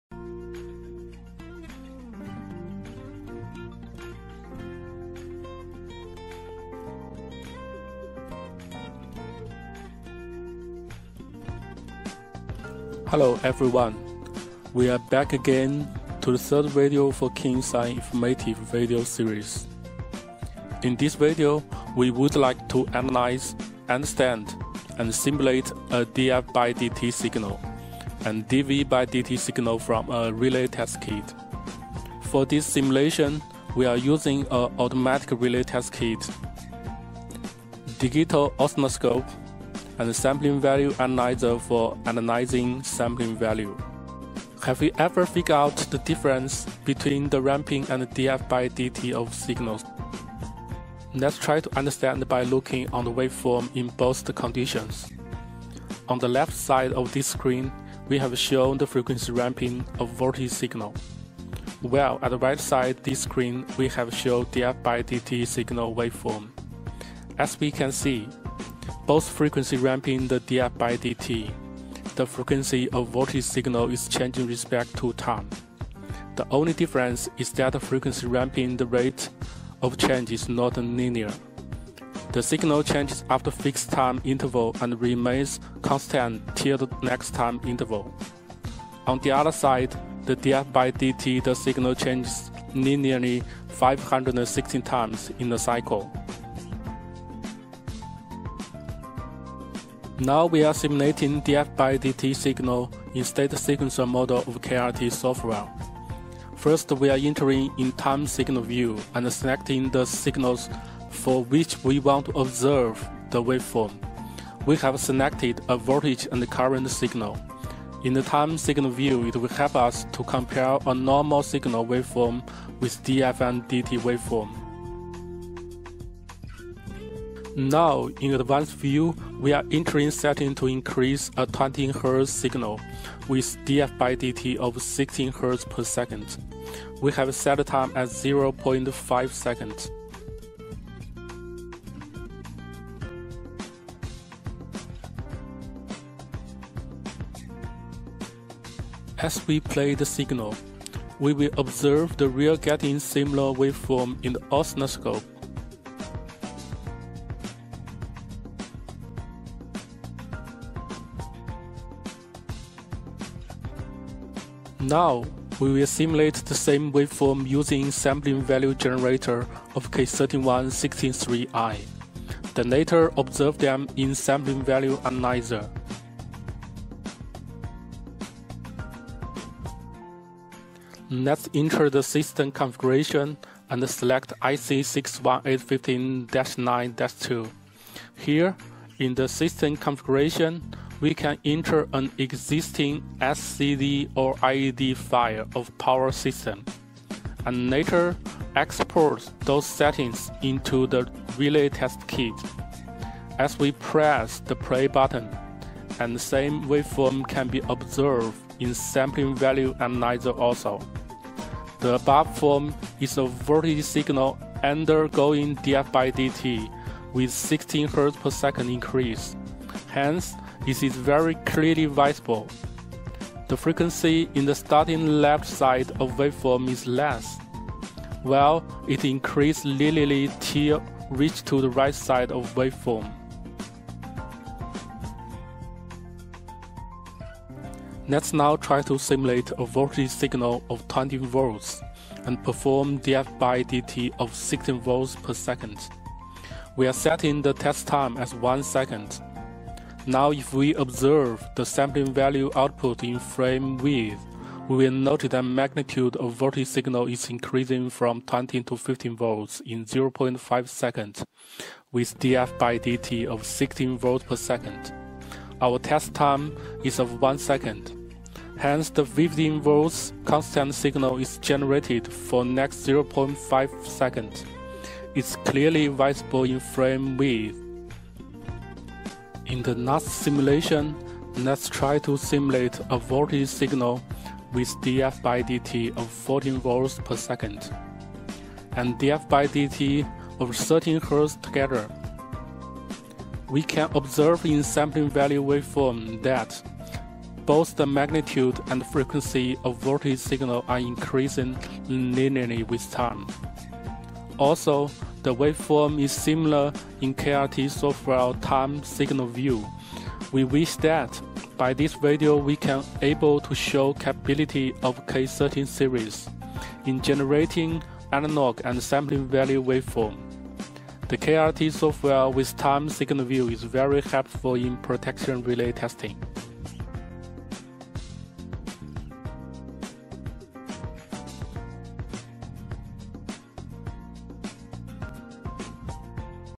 K3163i Kingsine relay test kit - Test df/dt, dv/dt , (ROCOF) in Analog and IEC61850 SV mode
KMS3, demo for dv/dt, df/dt, frequency test with Kingsine's KRT software